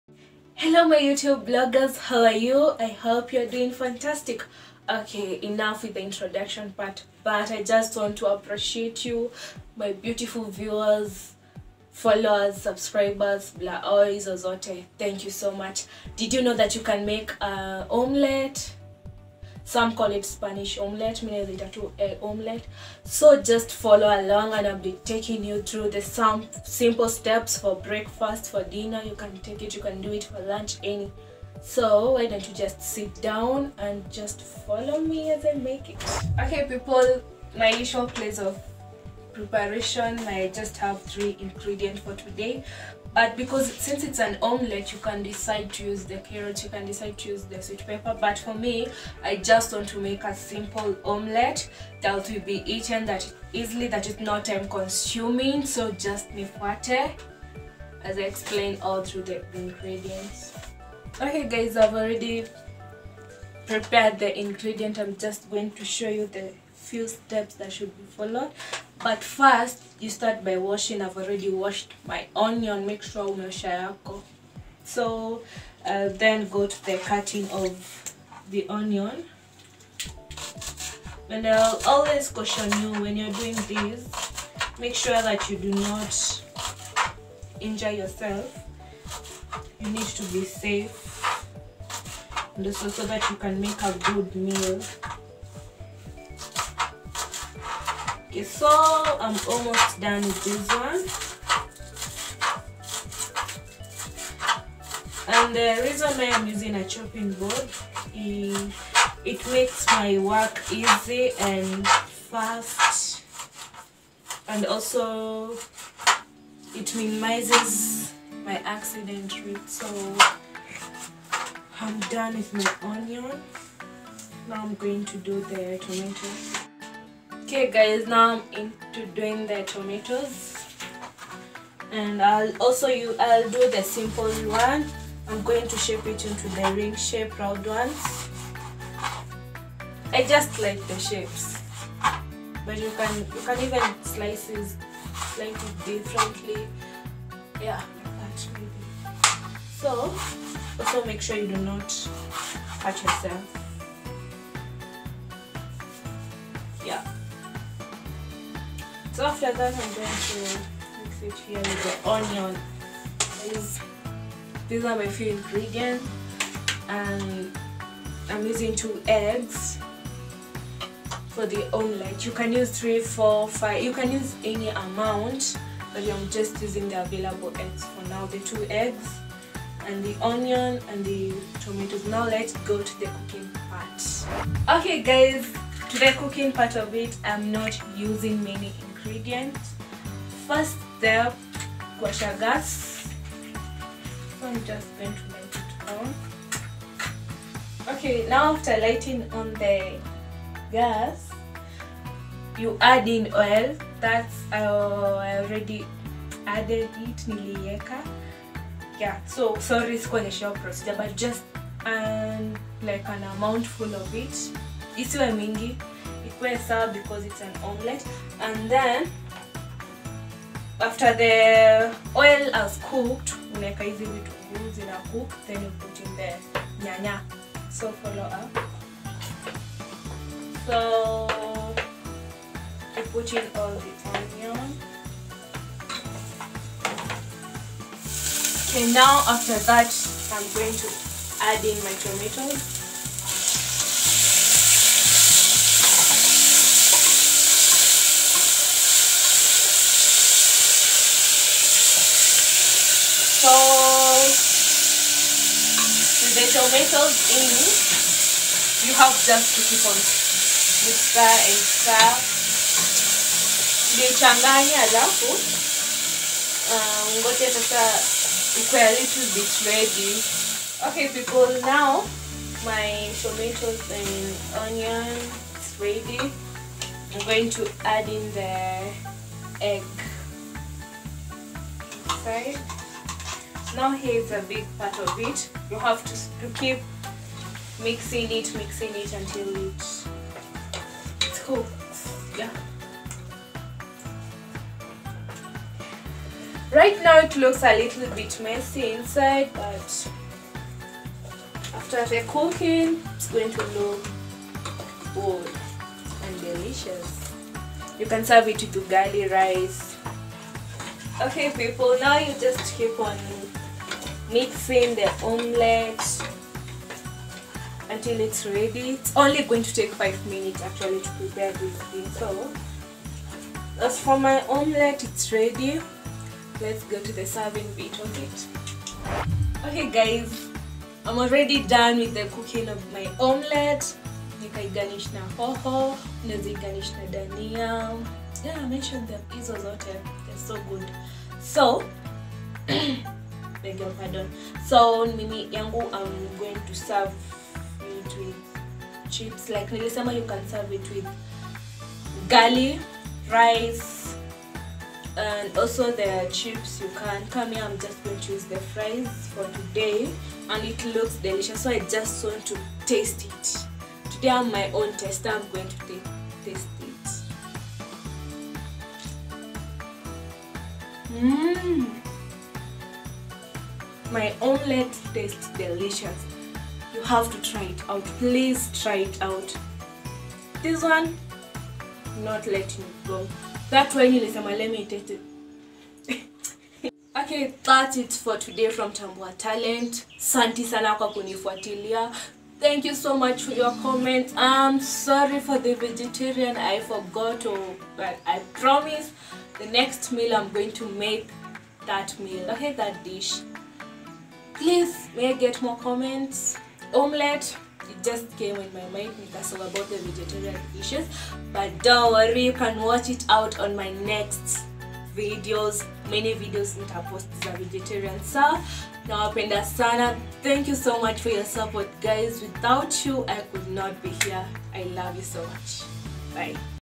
ONE EASY AND UNIQUE WAY TO MAKE AN OMELETTE AT HOME
in this video am making omelette in different way that you did not know.
please follow me and am sure you will like it
give us a thumb up if you liked the video.